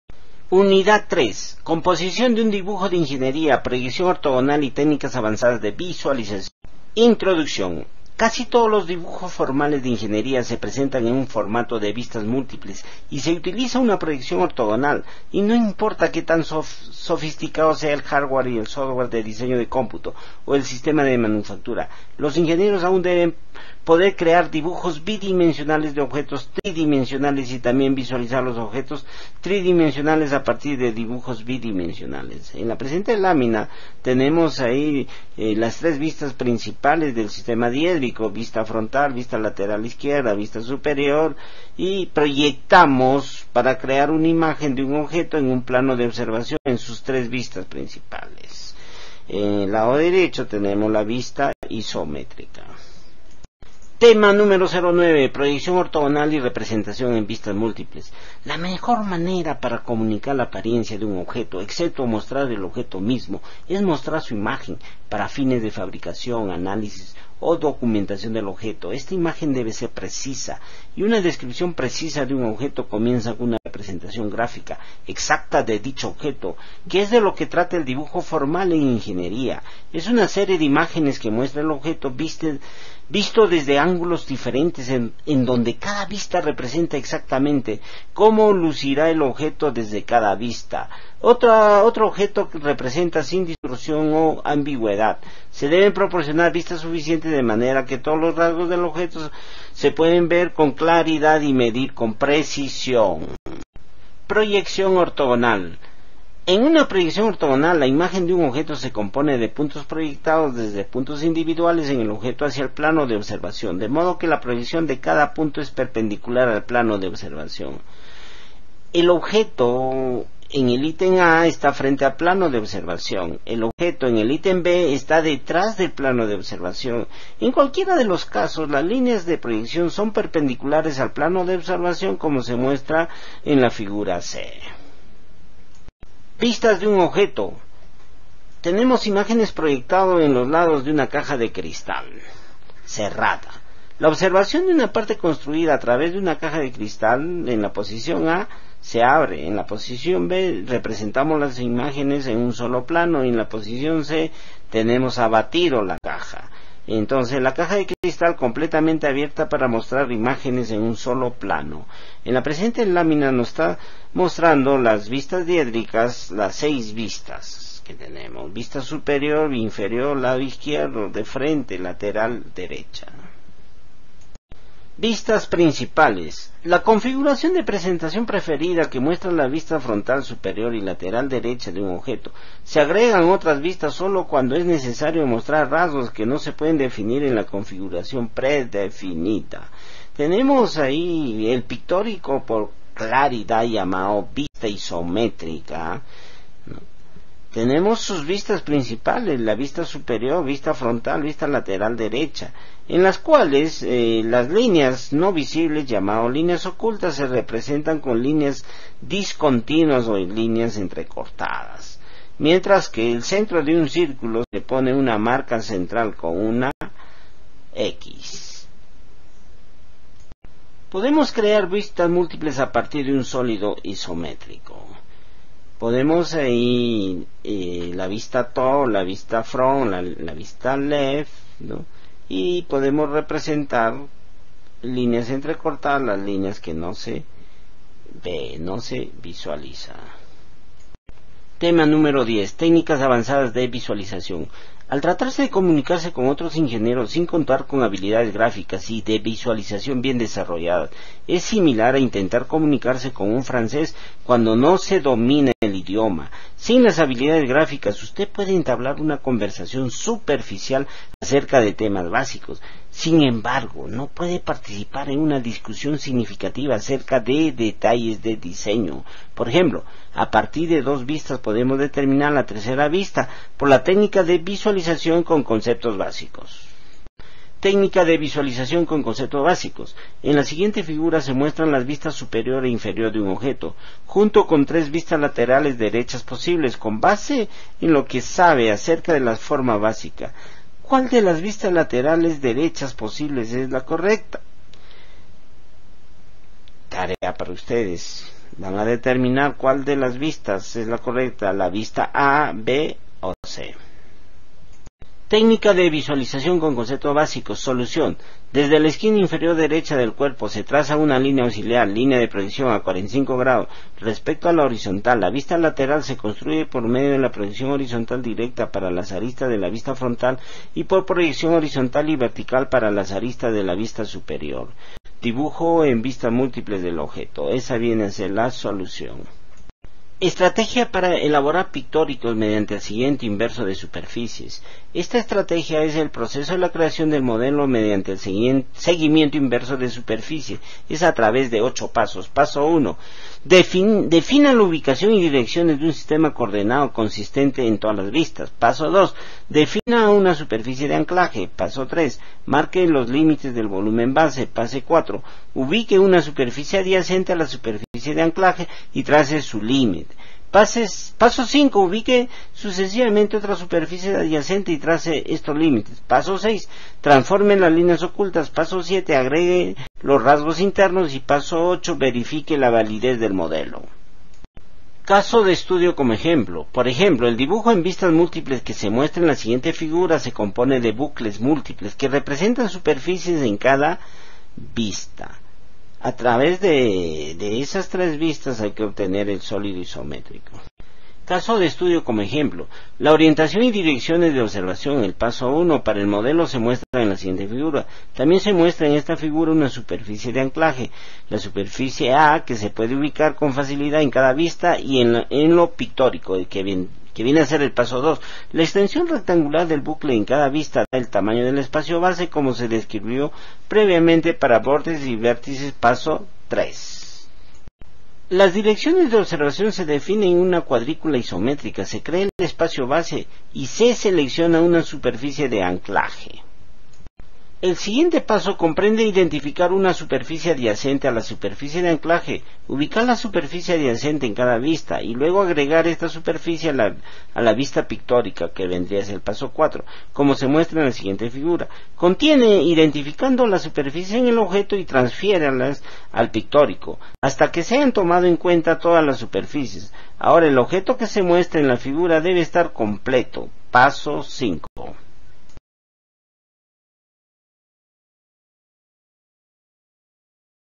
Composición de un dibujo de ingeniería: Proyección ortogonal y técnicas avanzadas de visualización
Dibujo para Ingeniería